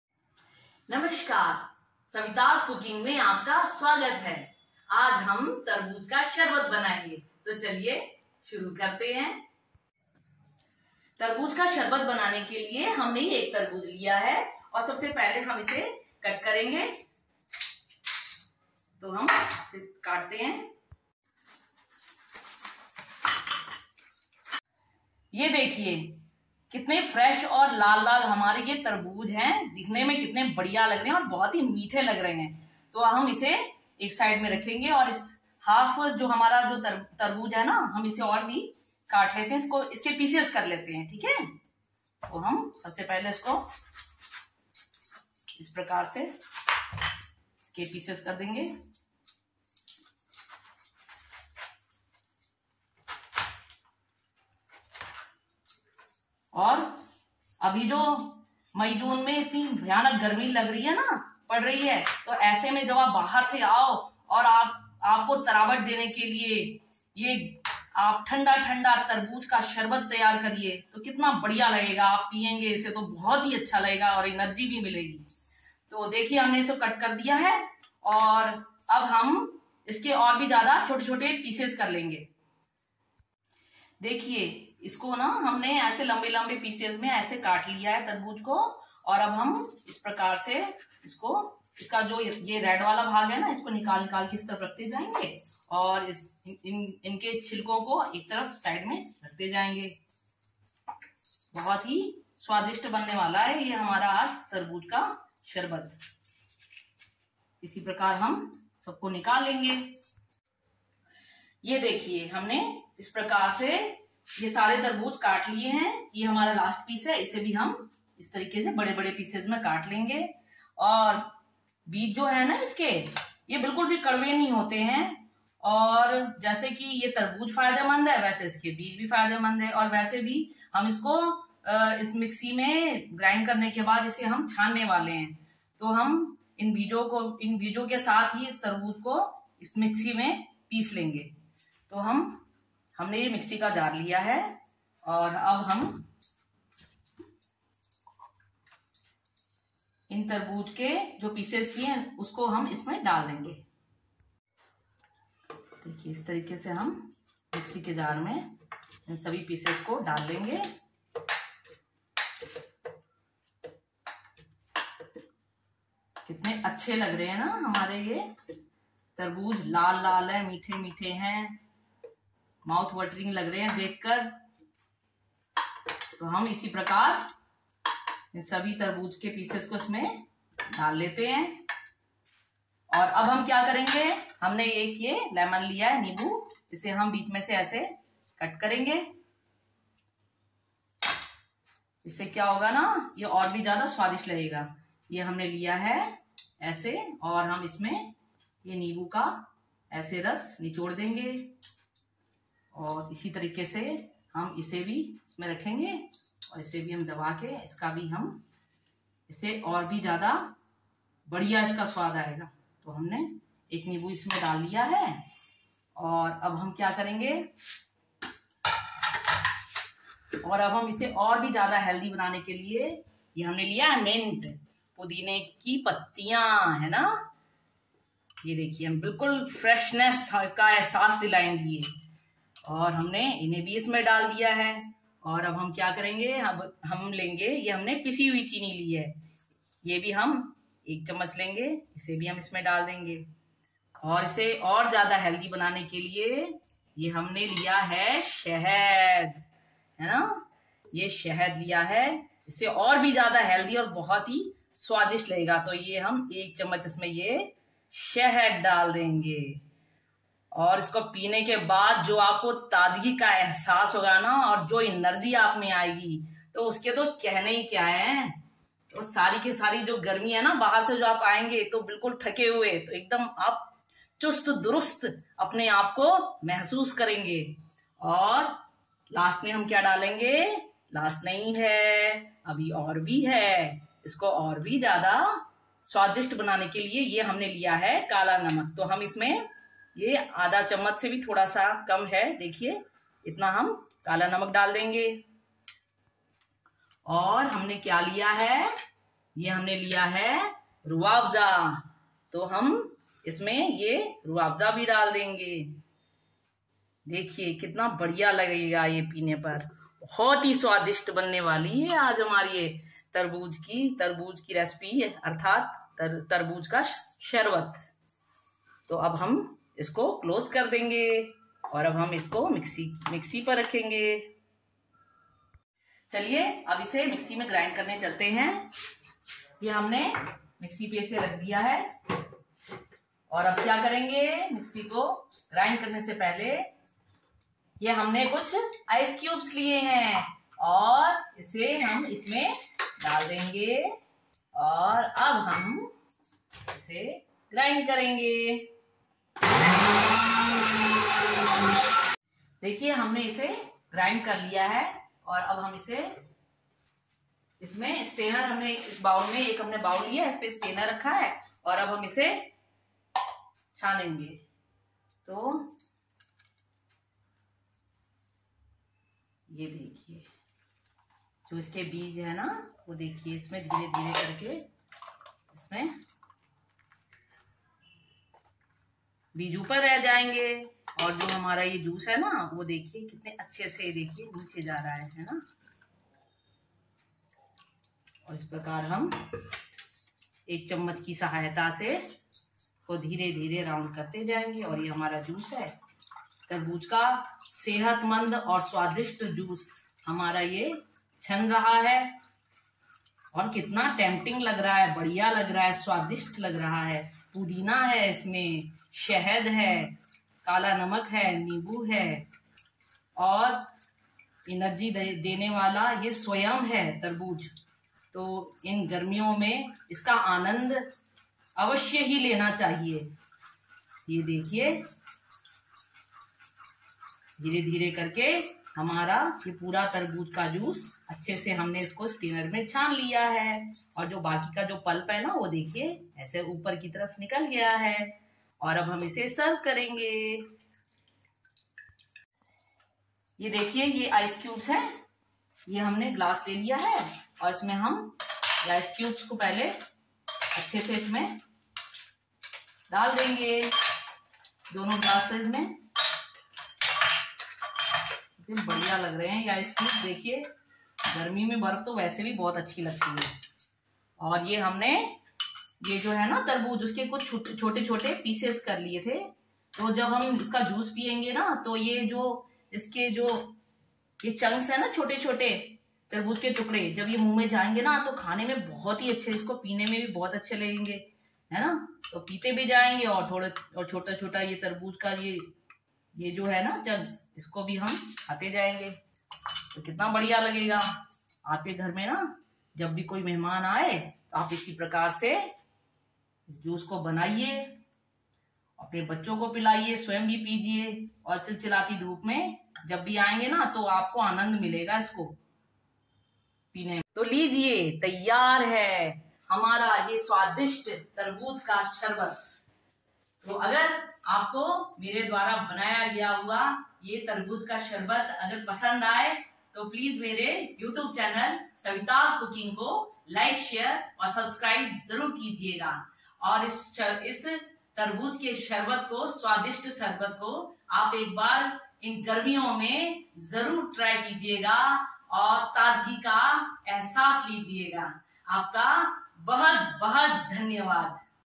Refreshing Homemade watermelon juice| तरबूज का शरबत|Health Benefits Of Watermelon| bySavitas Cooking
Refreshing Homemade watermelon juice| तरबूज का शरबत|Health Benefits Of Watermelon| bySavitas Cooking 🤞😋savitascooking